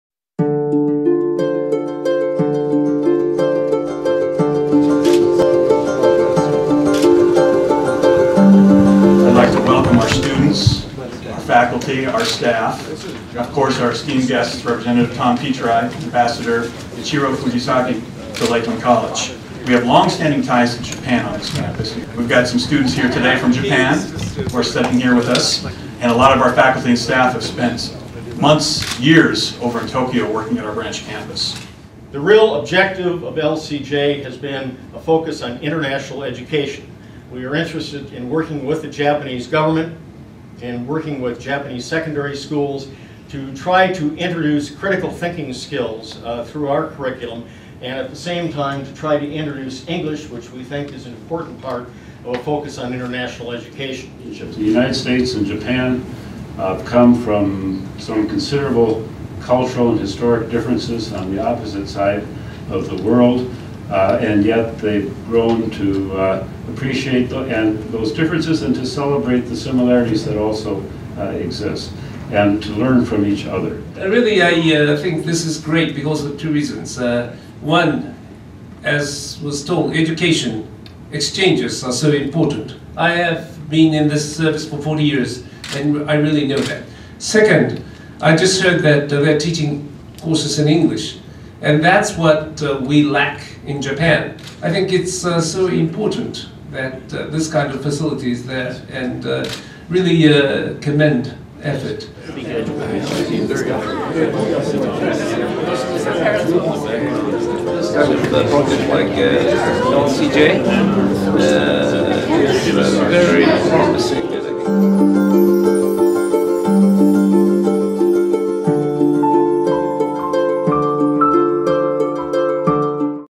Japanese Ambassador visits Lakeland College
Japanese Ambassador Ichiro Fujisaki lauded Lakeland College's two-year campus in Tokyo during a visit with U.S. Rep. Tom Petri at Lakeland's primary campus in Sheboygan County on Nov. 4, 2010.